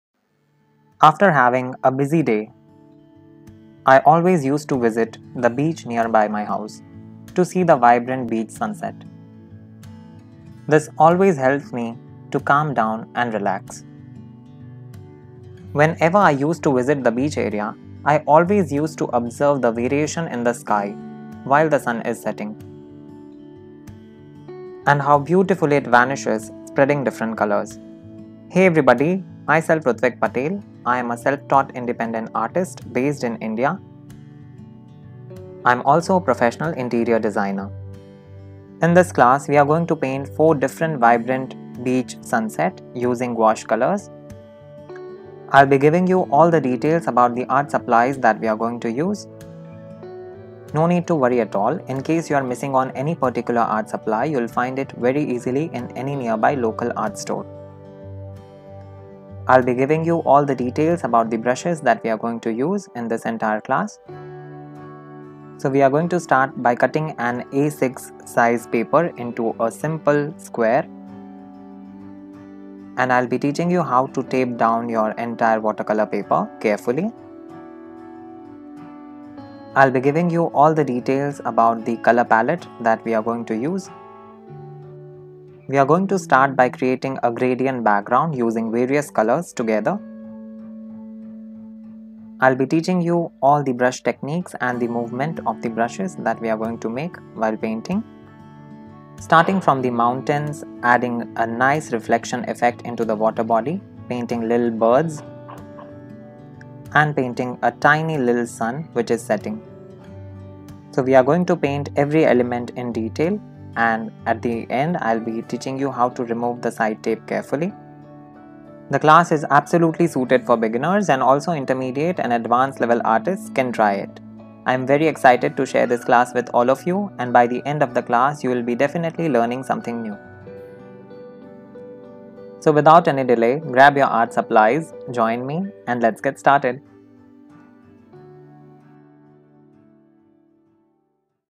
Vibrant Beach Sunset Painting Introduction to new skillshare class
Hello dear students, you are most welcome to my new class - Vibrant Beach Sunset Painting - Four Easy Gouache Paintings for Beginners.

Painting vibrant beach sunset using the medium of gouache colors is really a great experience. In this class we are going to learn how to paint four beautiful Vibrant Beach Sunset painting with easy and simple painting techniques.

Topics that we are going to learn in this class :
Details about all the art supplies that we are going to use
How to cut A4 size watercolor paper into four equal square parts
How to tape your watercolor paper from the sides
How to paint a vibrant background
How to paint elements in contrast with the background
How to remove your side tape carefully
All the four paintings are easy, simple and elegant to create and it won't take more the 10-20 minutes to paint. So you can paint according to your time convenience. I am super excited to share this class with all of you, so without any delay grab your art supplies and join me. Happy Painting.

Art Supplies you will need :
Anupam watercolor paper 140 lb,
Artist grade gouache colors from brustro ( you can use any other good alternative as well )
A pair of scissor
1" masking tape
30 cm scale
Brushes -  round brush size - 1, flat brush size 1" and size 6
One ceramic cup for water
Paper towel for dabbing your brushes
A palette to mix your paints
A pencil and an eraser
This class is best suited for all level artists. I am super excited to share this class with all of you and without any delay grab your art supplies and join me. Thanks a lot.